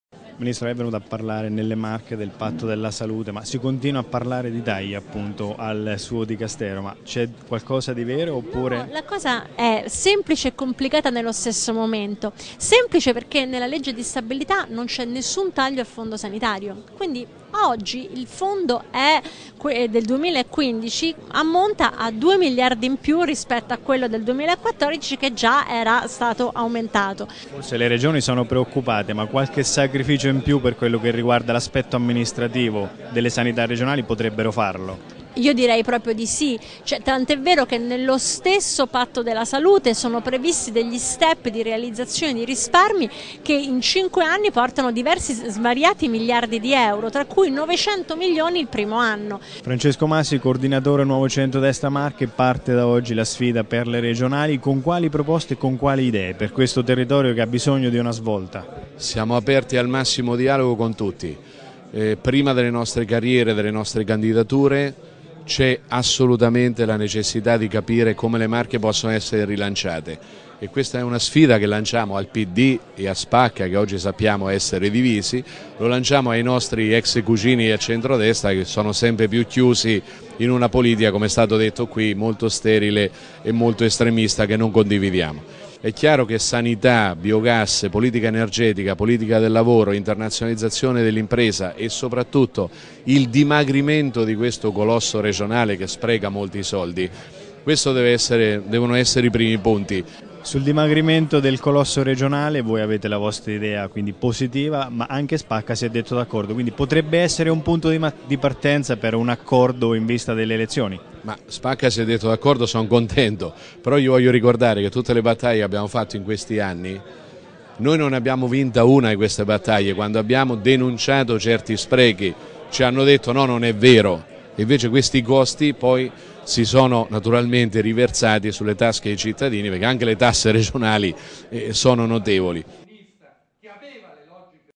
NCD Marche, la sfida per le elezioni regionali parte dalla sanità
Parliamo di politica e di elezioni regionali. Parte dalla salute la sfida del Nuovo CentroDestra Marche per l'appuntamento elettorale del 2015. In un incontro svoltosi a Civitanova, con il ministro Lorenzin e il coordinatore Quagliariello, il partito di Alfano ha parlato di sanità e di possibili alleanze per la tornata amministrativa, che toccherà anche le Marche.